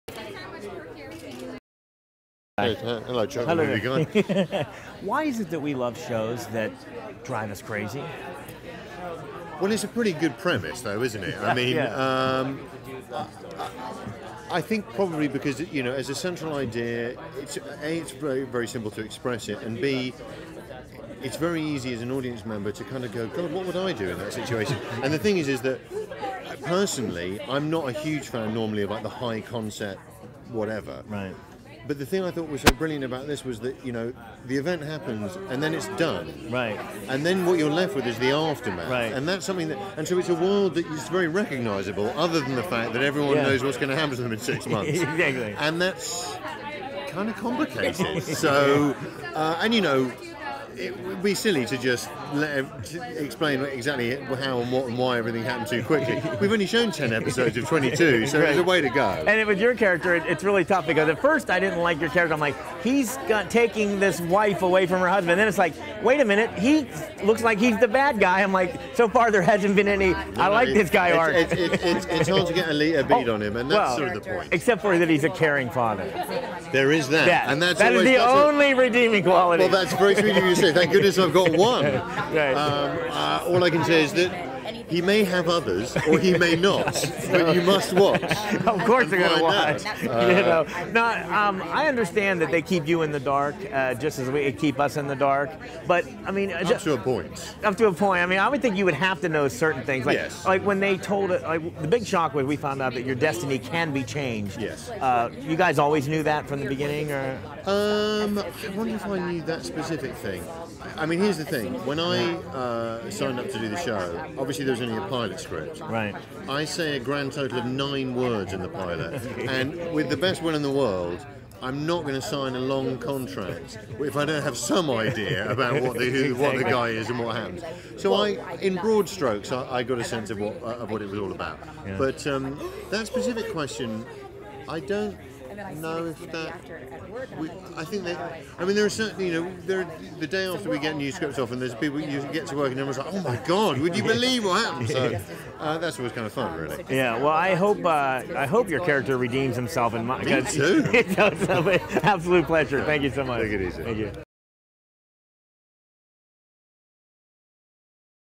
Jack Davenport interview for Flash Forward at the Paleyfest TV Festival 2010
Chuck the Movieguy interviews Jack Davenport who plays Lloyd Simcoe on Flash Forward at the William S. Paley Paleyfest TV Festival.

For more information about Paleyfest or the Paley Center for Media